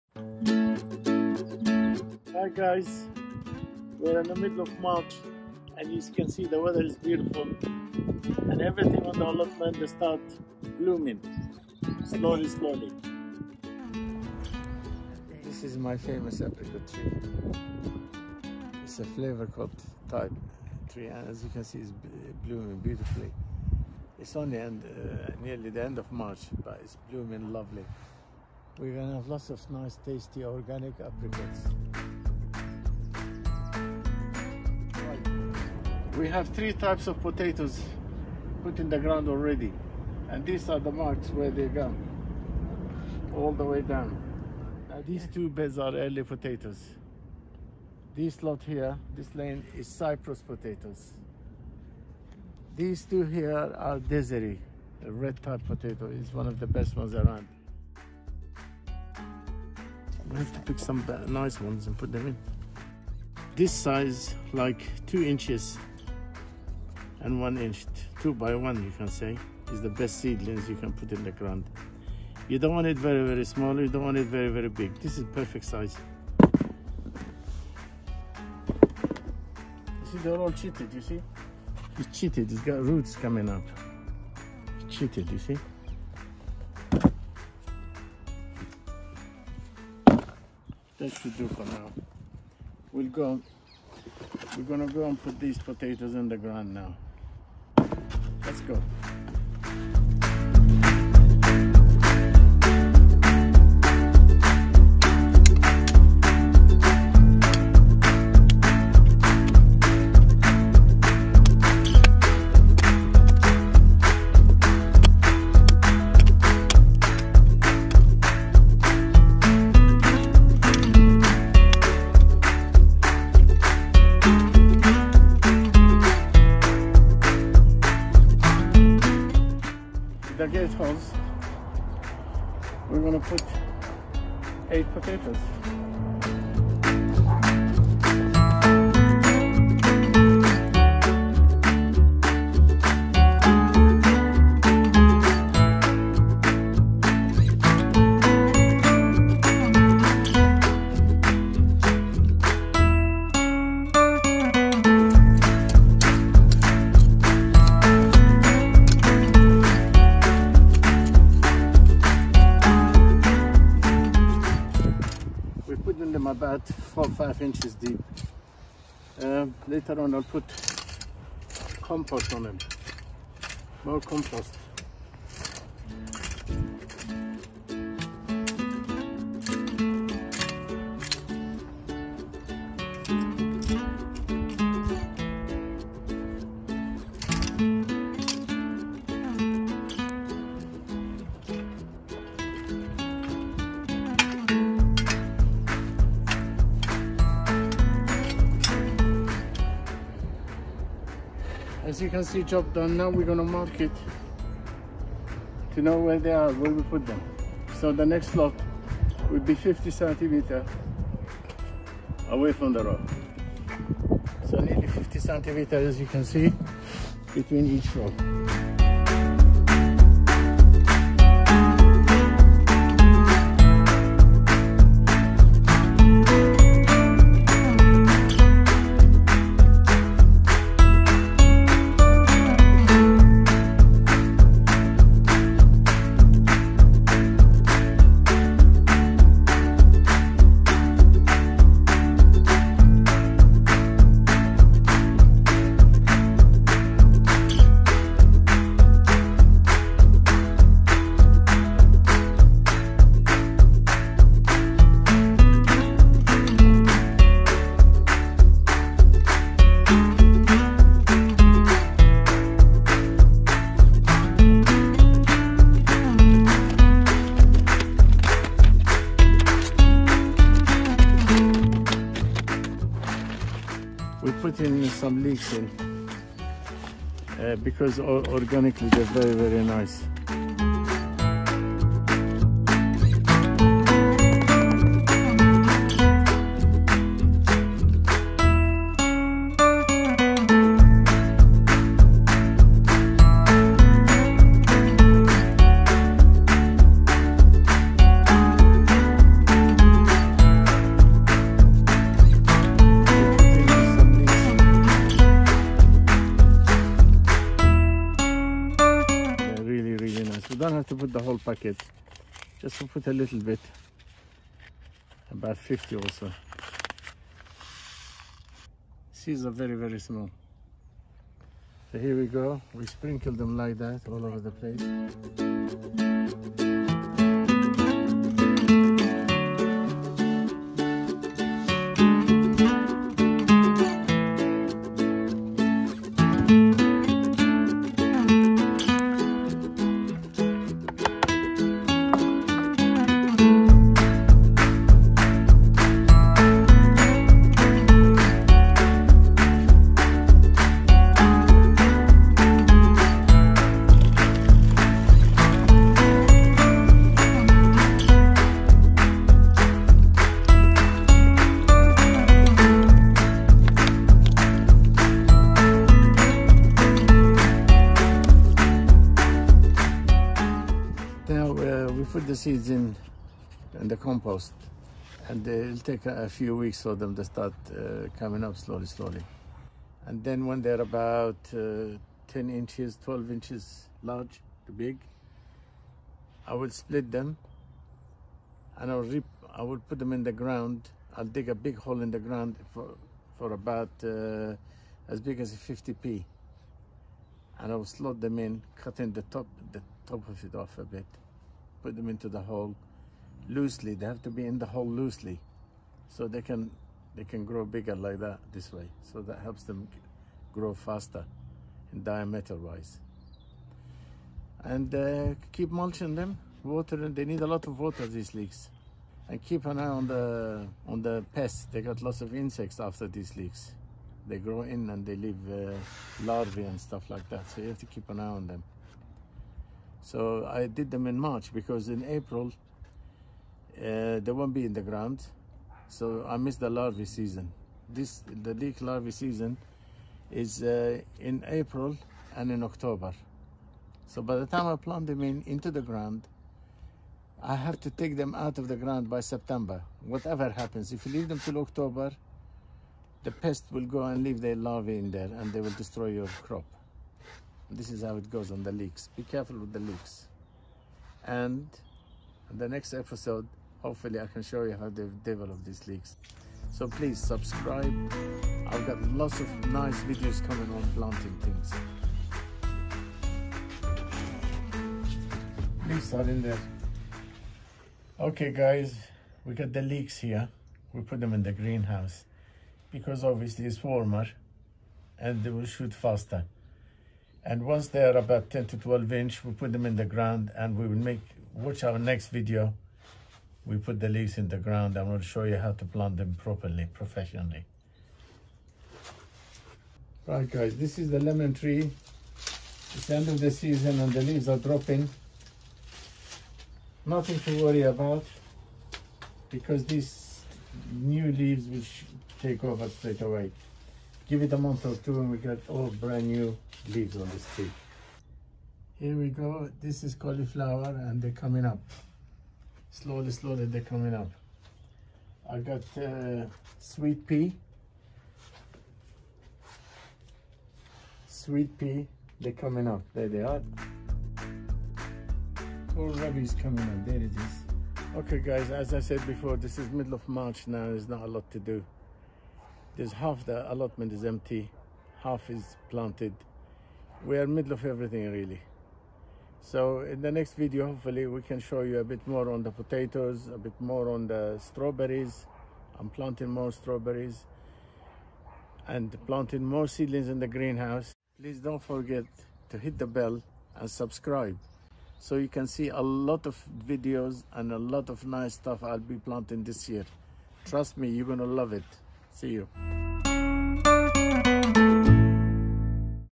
Planting Potatoes and Leeks in March at the Allotment with Mo
Mo shows us around his allotment in March. Half of the beds are planted, and half are empty. We see tulips, daffodils, apricot blossom, garlic and runner beans coming up in the ground.  He plants a row of Desiree potatoes, and a pot of leeks. In the greenhouse, the lemon tree is losing its leaves, but the planted seedlings are beginning to grow - we see cauliflower, sweet pea and kohl rabi shoots coming through. The weather is lovely and sunny, though still fairly cold (around 11 degrees).  Enjoy watching Mo work his plot!